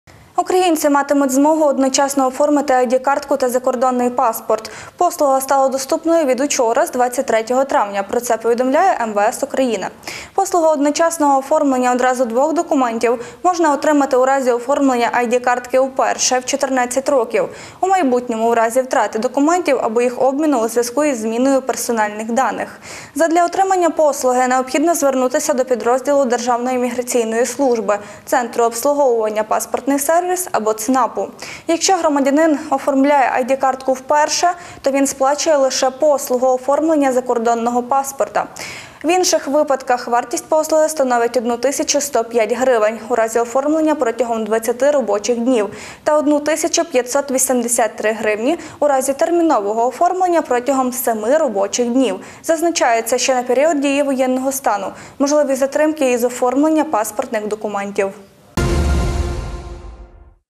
Спрощена послуга оформлення документів. Доступна нова послуга!
Українці матимуть змогу одночасно оформили ID-картку та закордонний паспорт – послуга стала доступною від учора, з 23 травня. Про це повідомляє МВС України. Послугу одночасного оформлення одразу двох документів можна отримати у разі оформлення ID-картки вперше в 14 років. У майбутньому - у разі втрати документів або їх обміну у зв‘язку із зміною персональних даних. Задля отримання послуги необхідно звернутися до підрозділу Державної Міграційної Сужби, центру обслуговування "Паспортний сервіс" або ЦНАПу. Якщо громадянин оформлює ID-картку вперше в 14 років, то він сплачує лише послугу оформлення закордонного паспорта. В усіх інших випадка, вартість послуги становить 1 105 грн (у разі оформлення протягом 20 робочих днів) та 1 583 грн (у разі термінового оформлення протягом 7 робочих днів). Зазначається, що на період дії воєнного стану можливі затримки із оформлення паспортних документів.